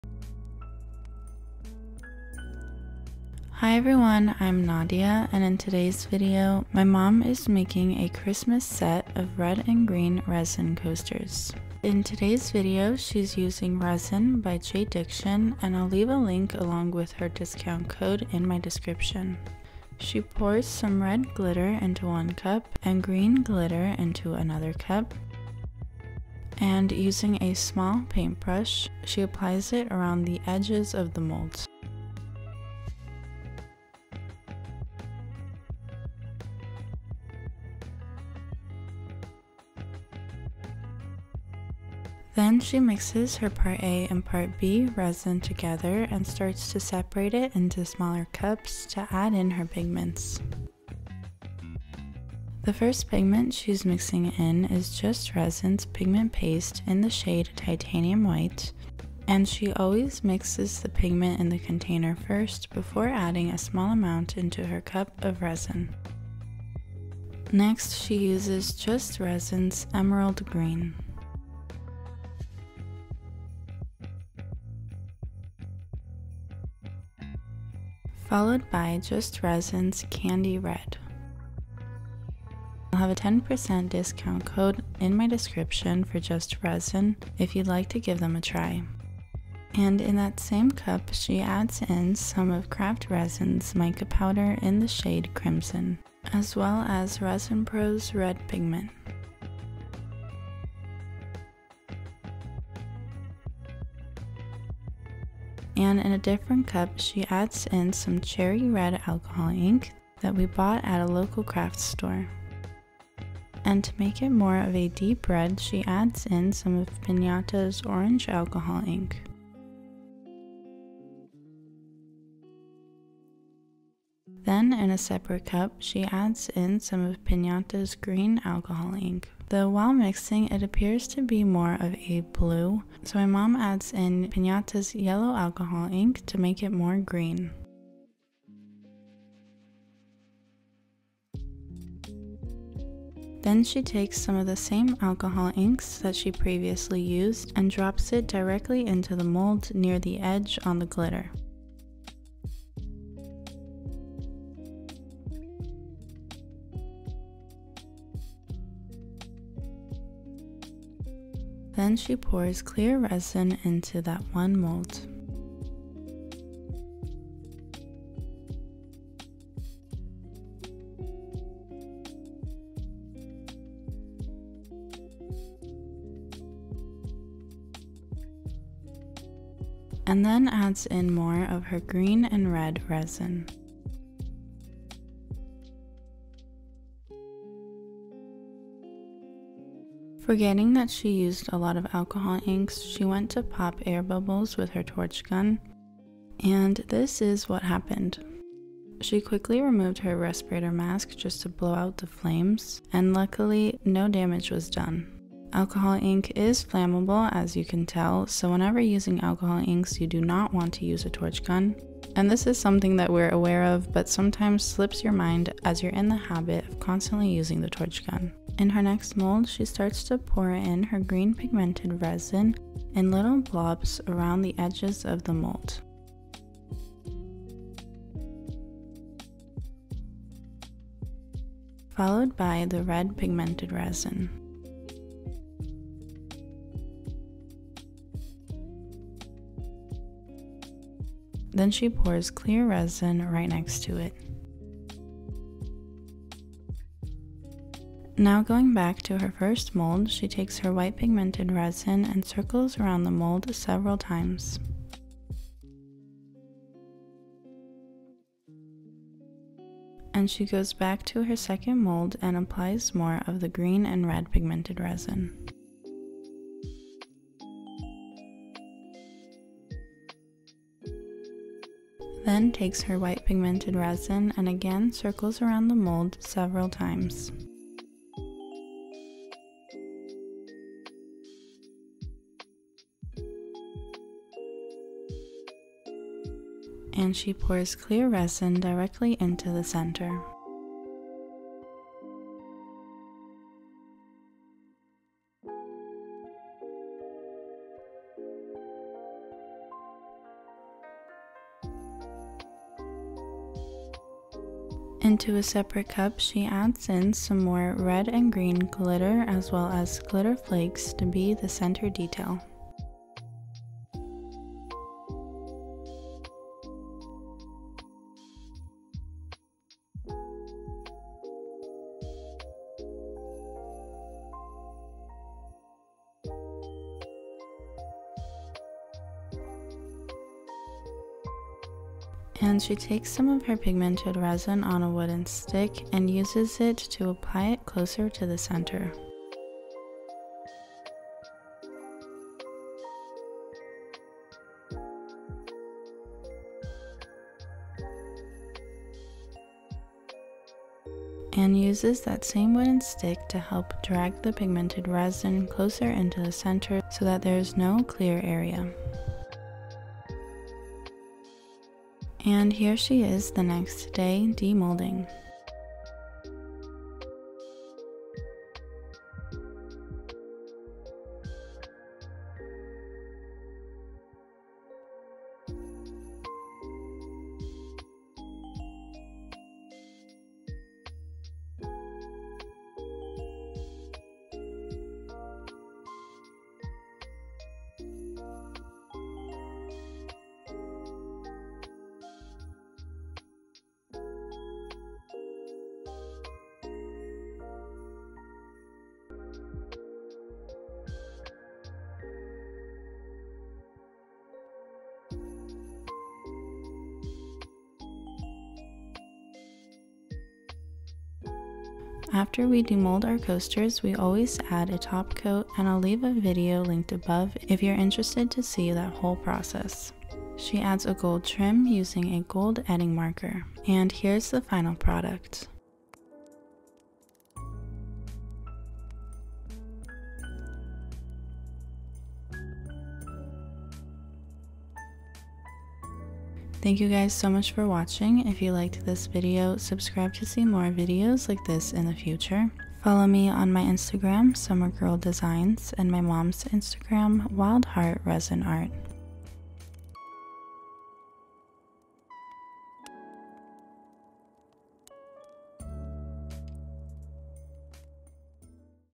Red & Green Christmas Inspired Resin Coasters
My mom makes a beautiful set of Christmas inspired resin coasters.

Etsy Shop

Here are the products we used! Some aren’t available but I have found similar products:

U.S:

Resin Coaster Molds
Green Glitter
Red Glitter
Alcohol Ink Set
Edding Gold Marker
Small Paper Cups
Butane Torch Gun
100 Pieces Jumbo Craft Sticks
Heat Gun 3-Temp Setting
Nitrile Disposable
Full Face Respirаtor